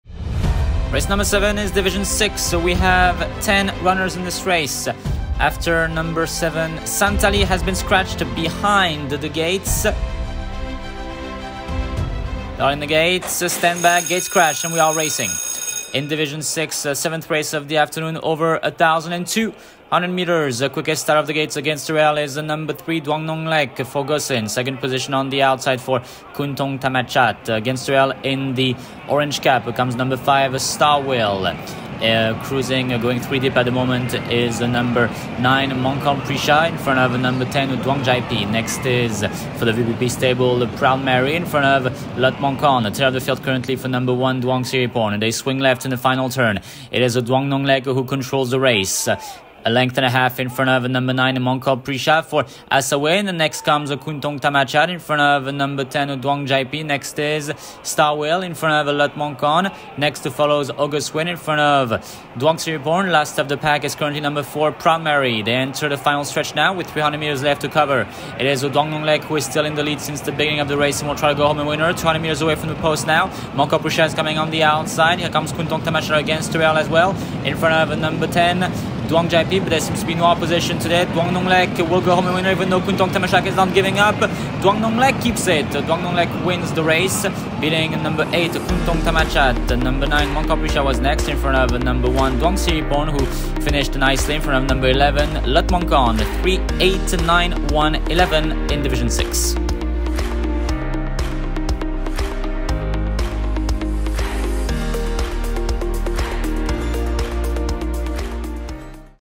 DUANG NONG LEK wins! - 31/03/2024 - Race 7 - Division 6
Bangkok, Thailand - DUANG NONG LEK wins! - 31/03/2024 - Race 7 - Division 6

Track going: Good to firm